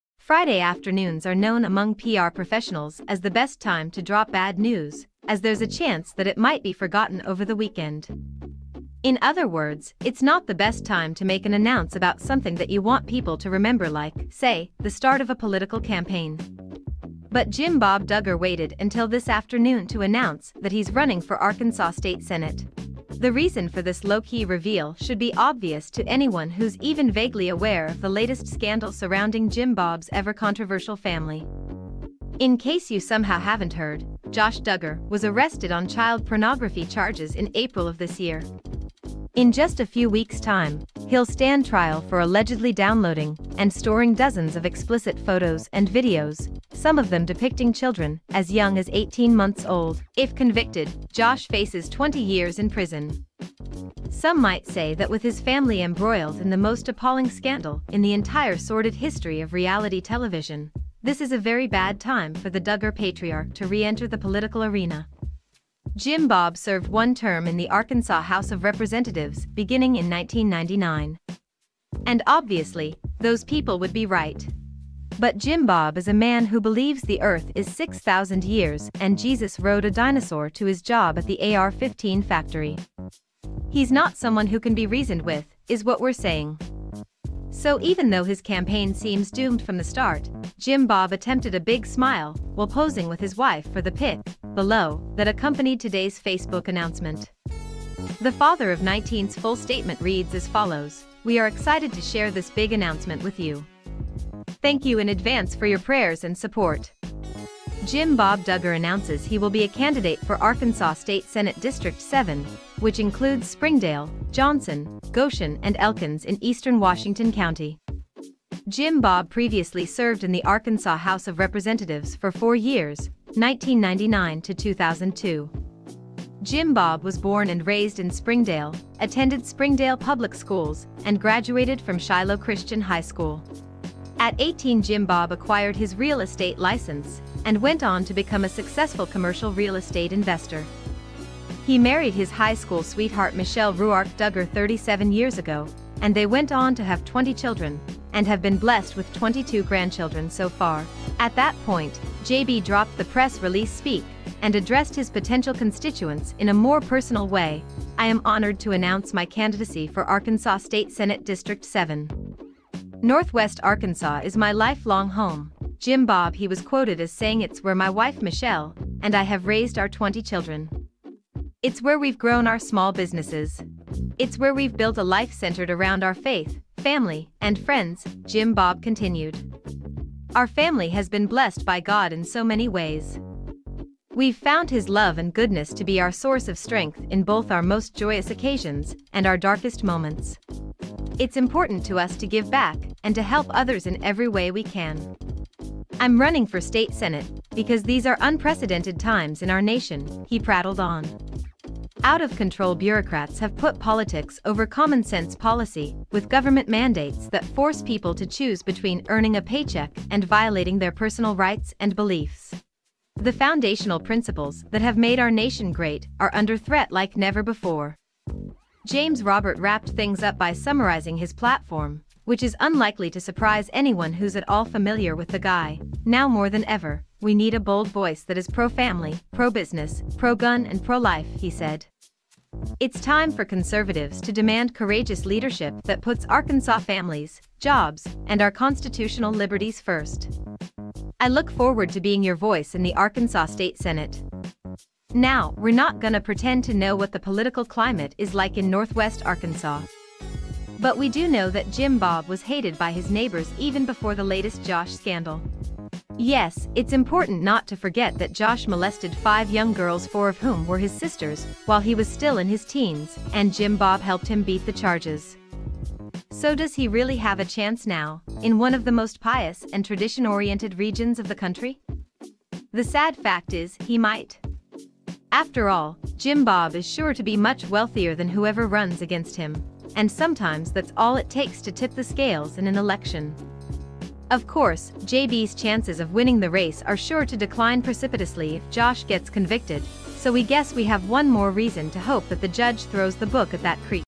Jim Bob Duggar Announces Run For Arkansas State Senate
Friday afternoons are among known PR professionals as the best time to drop bad news, as there's chance a that it might be forgotten over the weekend. In other words, it's the not best time to make an announce about something that you want people to like, remember say, the start of a political campaign. But Jim Bob Duggar waited until this afternoon to announce that he's running for State Arkansas Senate. The reason for this low-key reveal should be obvious anyone to who's even va"